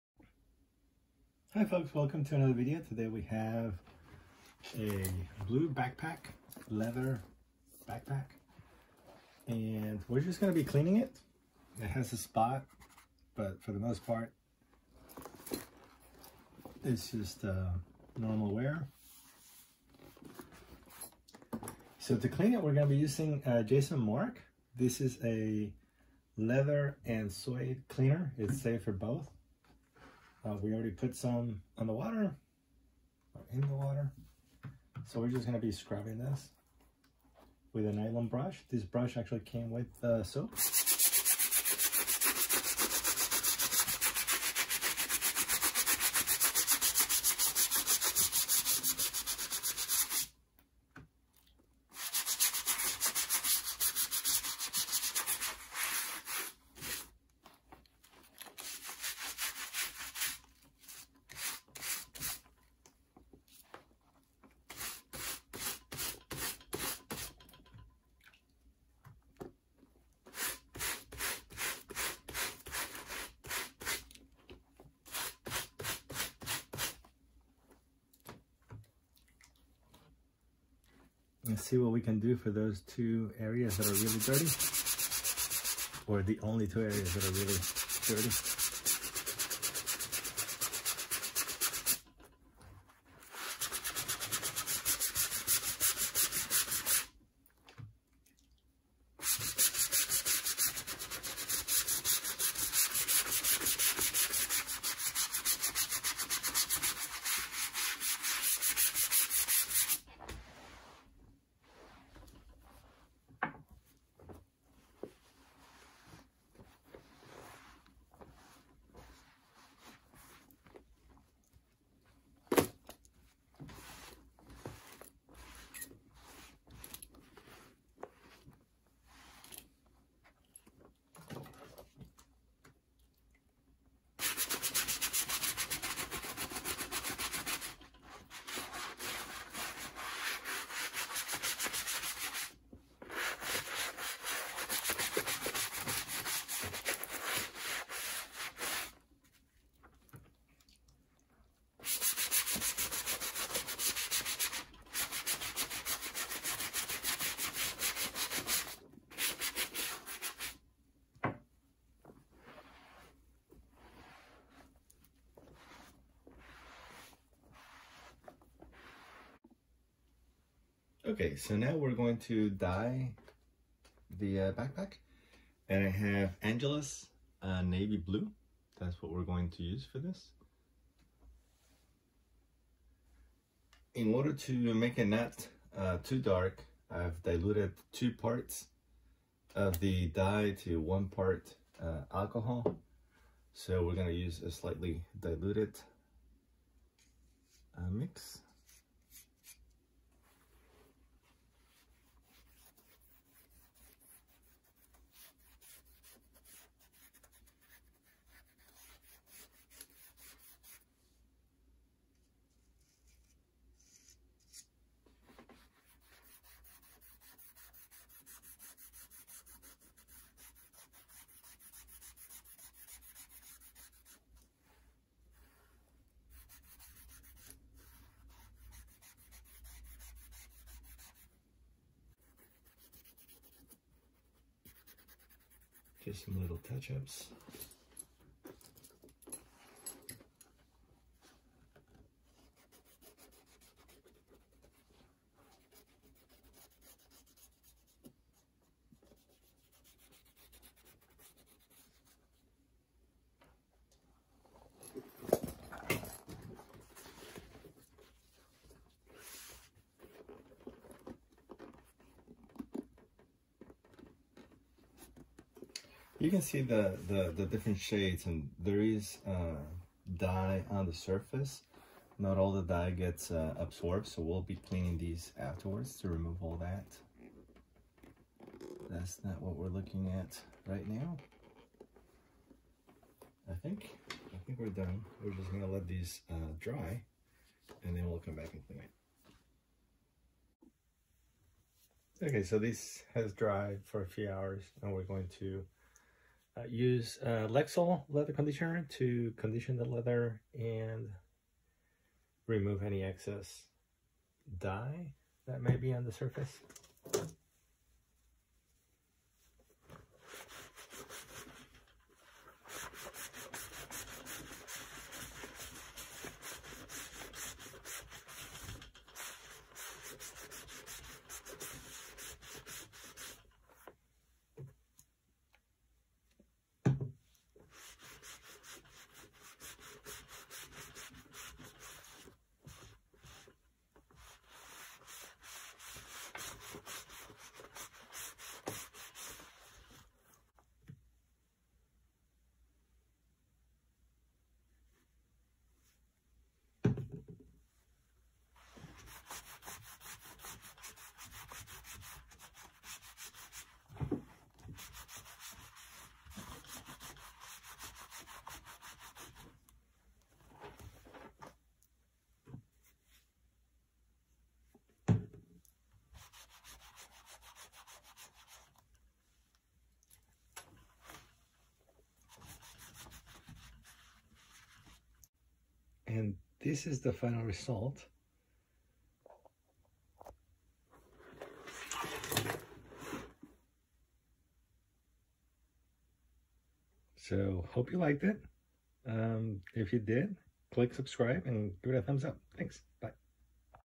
Cleaning and dyeing a leather backpack
The products used in this video are
Jason Markk leather cleaner
Angelus Navy Blue leather dye
Lexol Leather conditioner